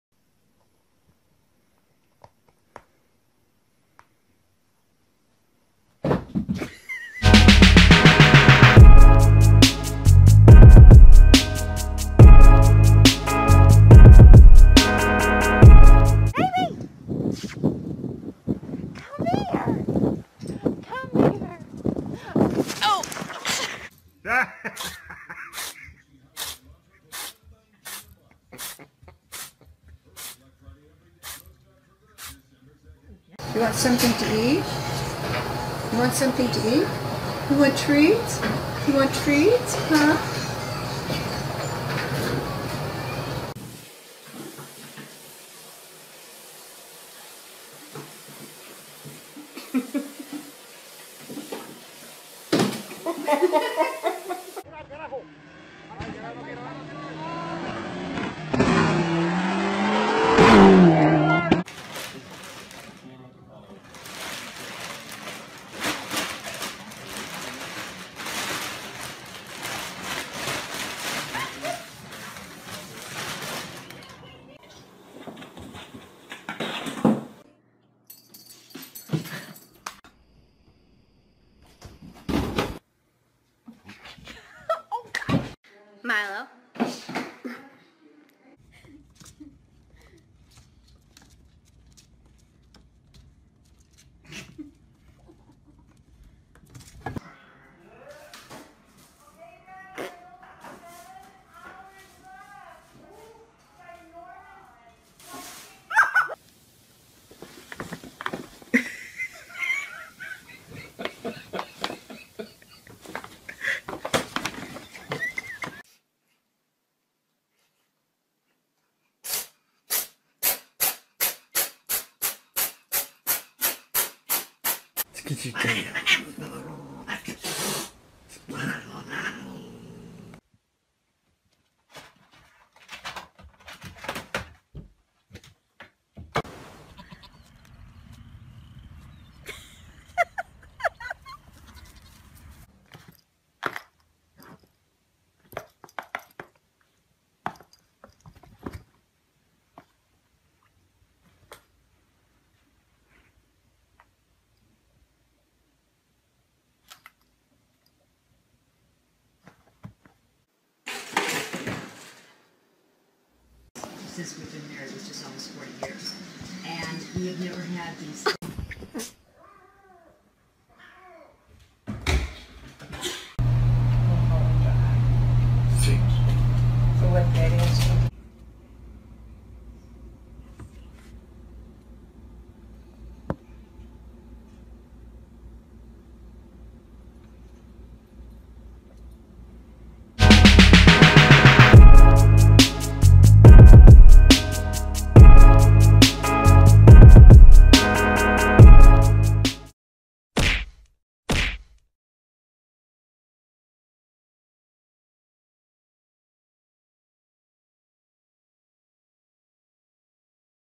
ANIMAL FAILS! #16
Best Animal Fails Compilation Part 16 for your viewing pleasure! Enjoy!